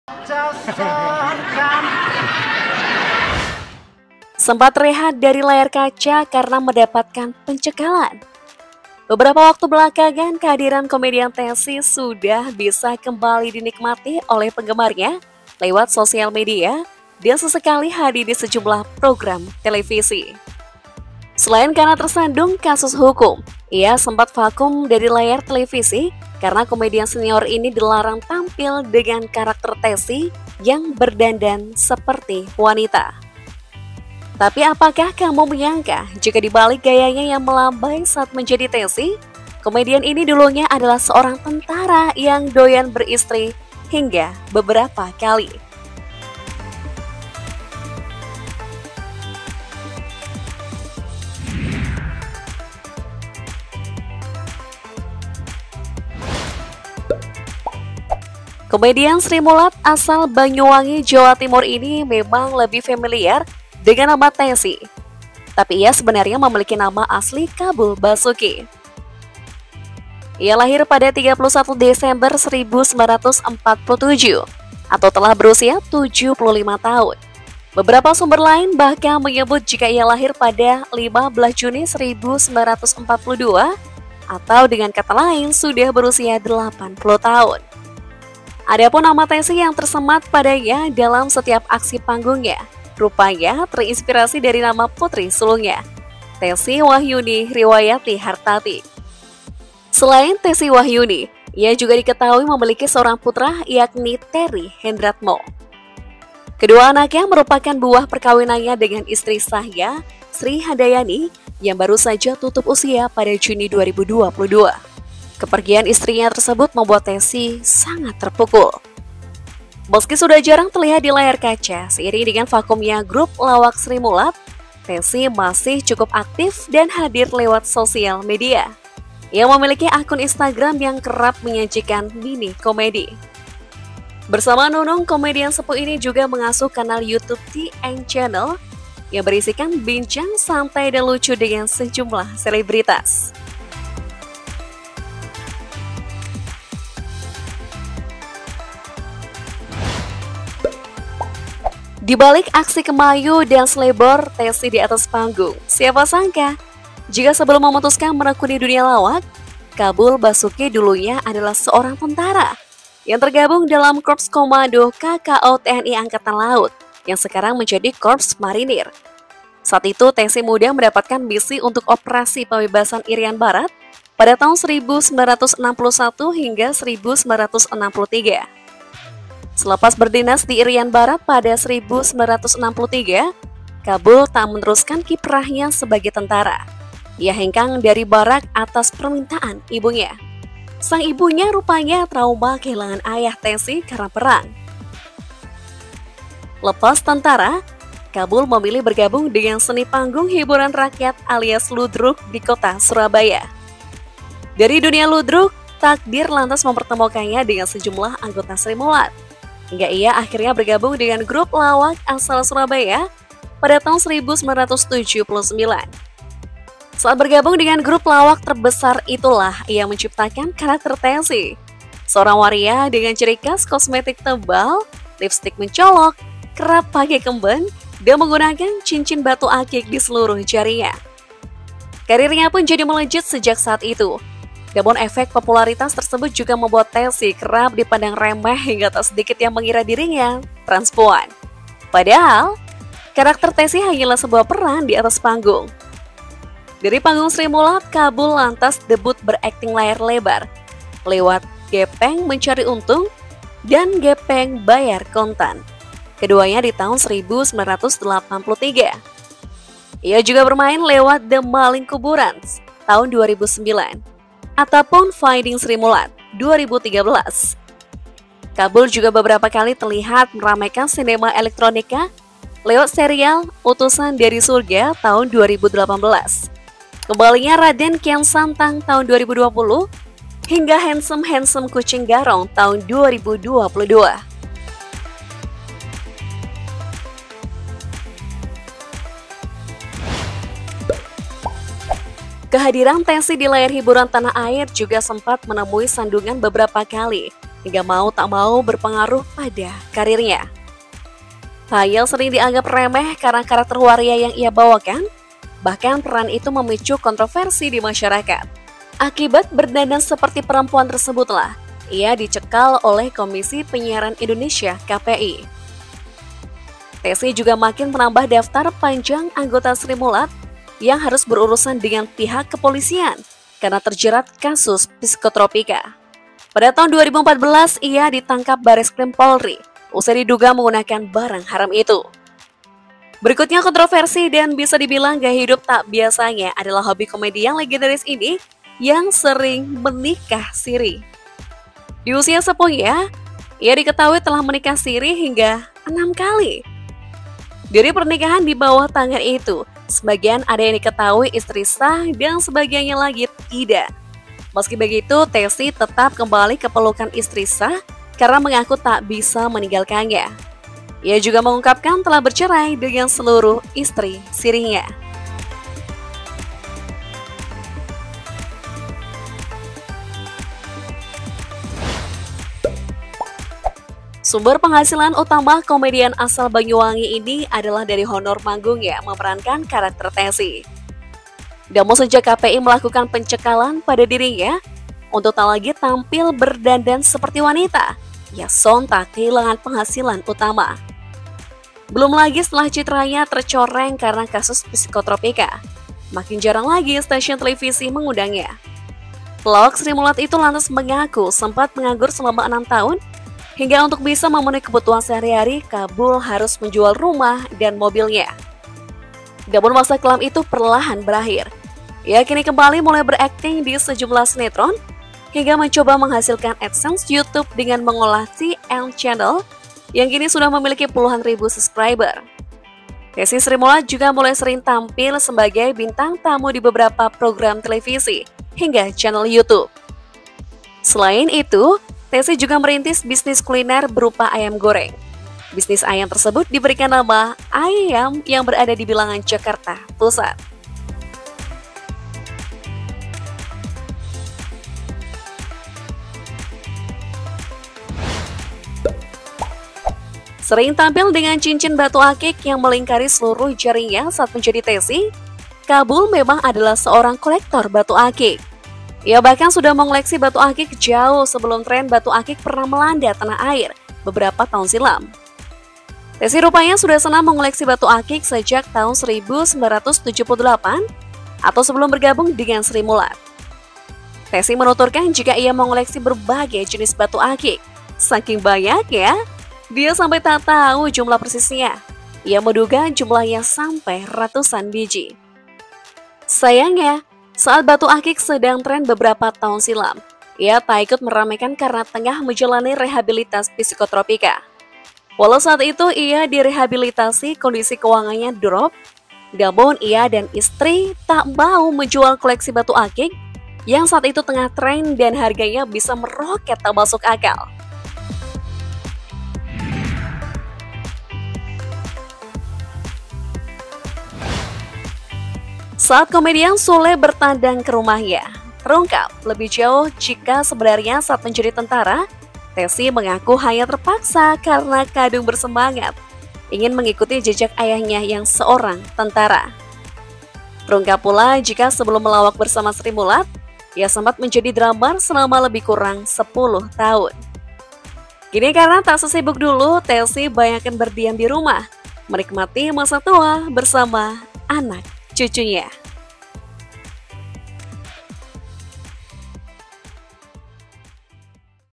NASIB MIRIS KOMEDIAN SENIOR? Begini Kabar Terbaru Tessy Srimulat, Kini Berubah Drastis
Selain karena tersandung kasus hukum, ia empat vakum dari layar televisi karena komedian senior ini dilarang tampil dengan karakter Tessy yang berdandan seperti wanita. Tapi apakah kamu menyangka jika dibalik gayanya yang melambai saat menjadi Tessy, komedian ini dulunya adalah seorang tentara yang doyan beristri hingga beberapakali.